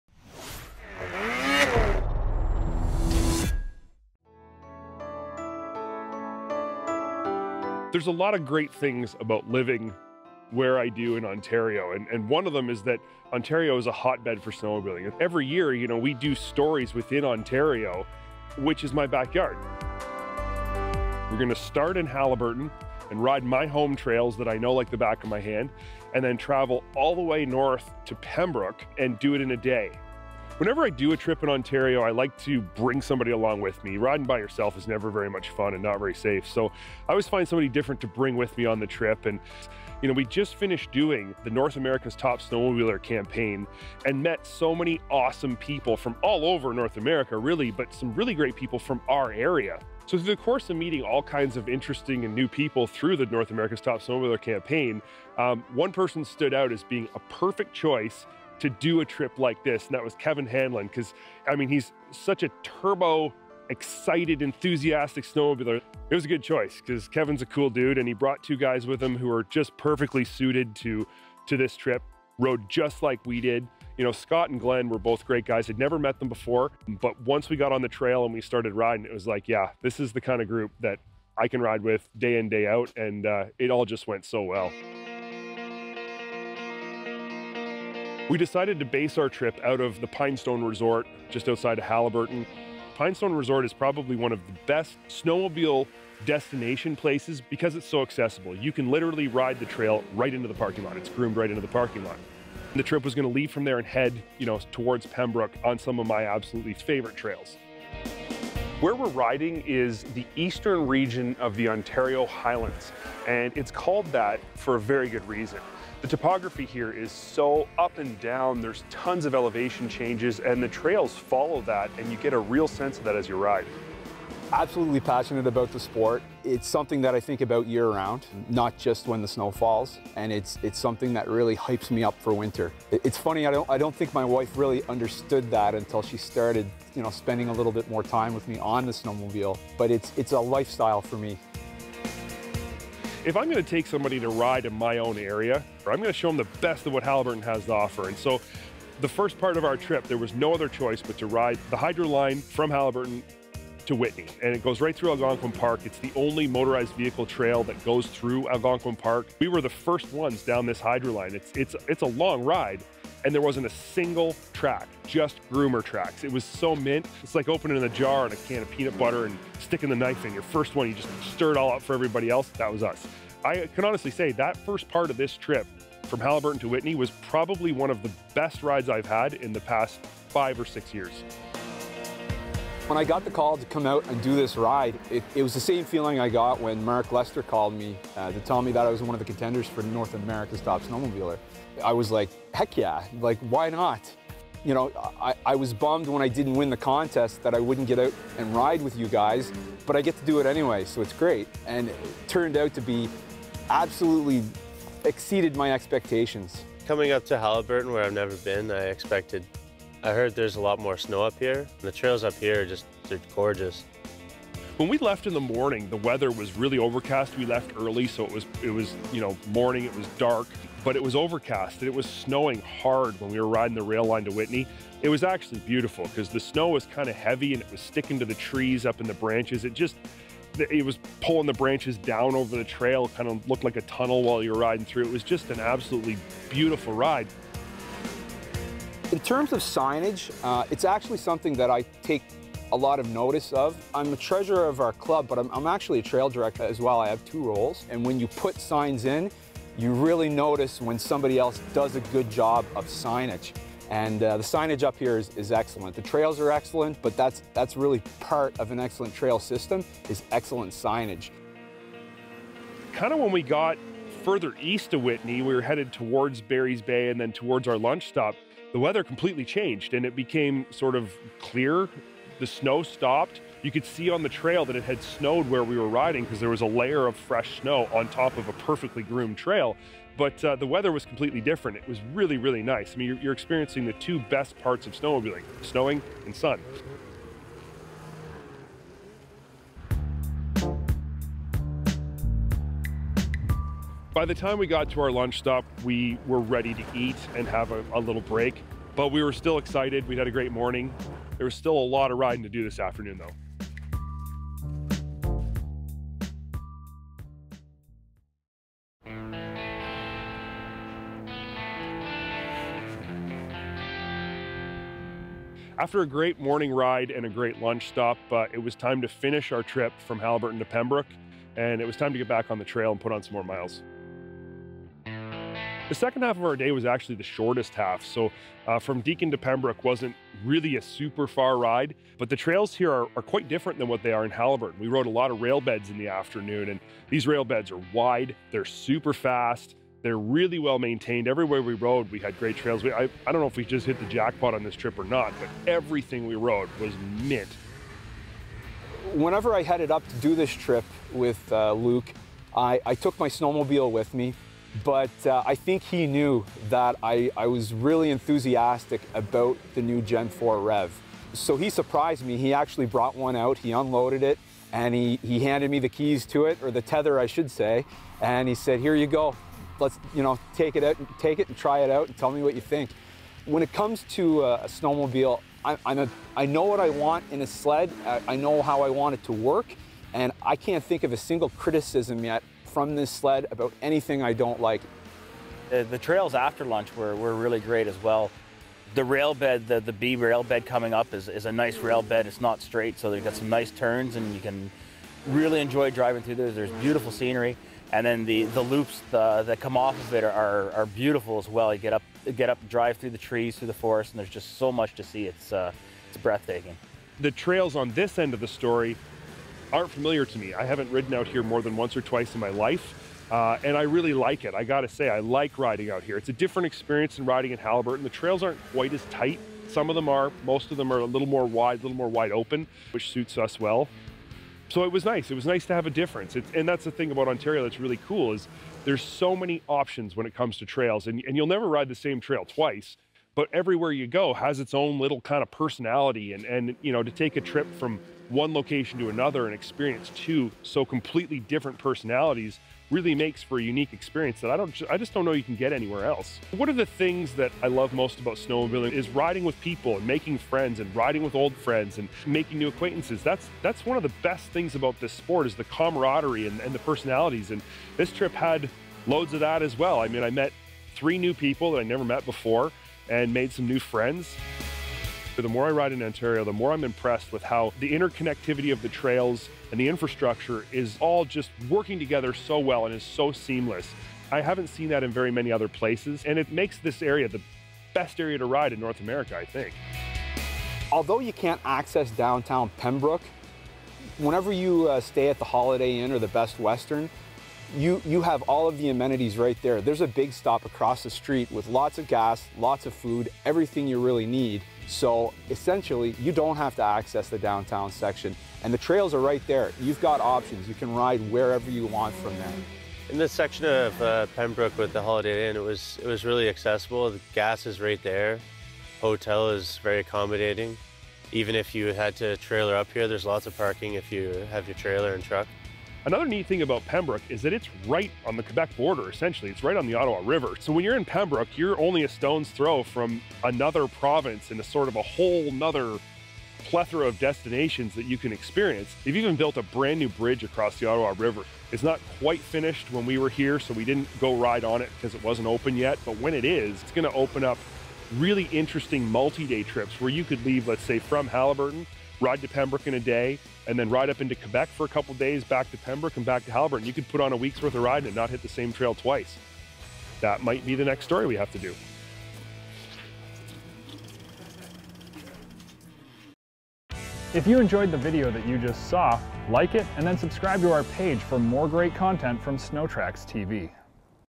Trail Riding In Ontario's Highlands
Luke’s riding out of his home turf in Ontario Canada on an adventure from Haliburton to Pembroke and back showcasing some of the beautiful trails Ontario's Highlands has to offer.

@onhighlands/Ontario’s Highlands, @ottawavalleytravel/Ottawa Valley and @Haliburton.Highlands/Haliburton Highlands and ComeWander.